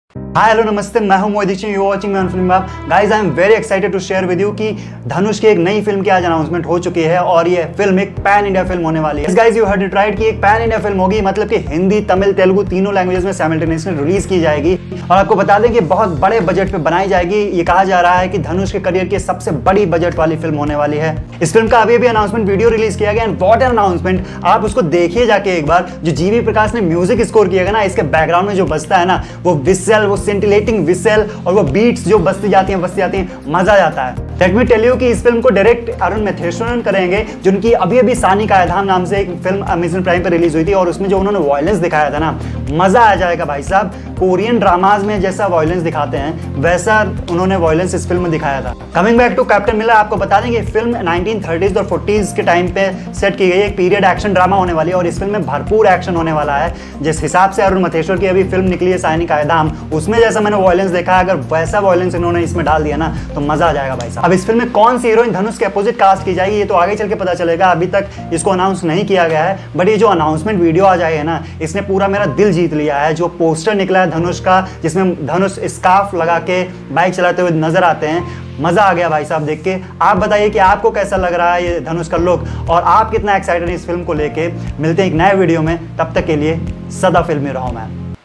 Dhanush As Captain Miller Will BEAT KGF? | 1st Pan India Film | Captain Miller Full Details
Actor Dhanush has announced his next film with Saani Kaaydham director Arun Matheswaran. Titled as Captain Miller, the film is high-budgeted action drama. Watch Out the full video for the details.

Follow Filmybaap Now.